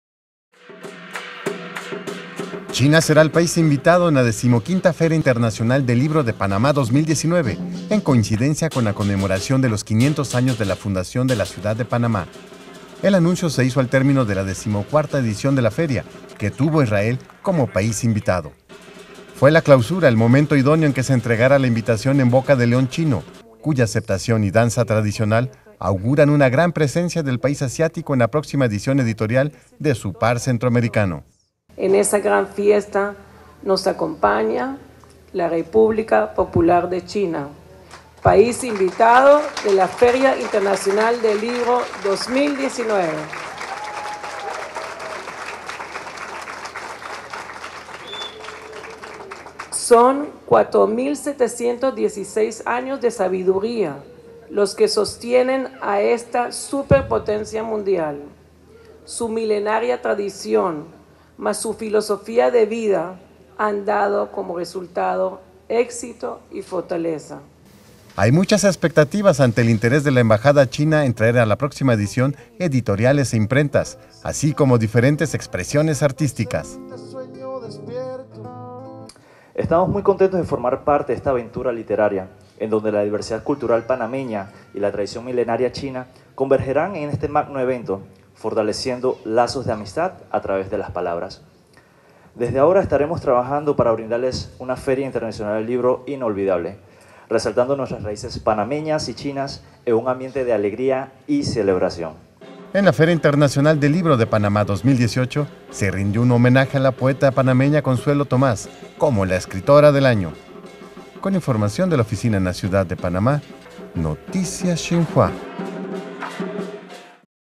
China, país invitado de la Feria del Libro Panamá 2019
China será el país invitado en la XV Feria Internacional del Libro de Panamá 2019, en coincidencia con la conmemoración de los 500 años de la fundación de la Ciudad de Panamá.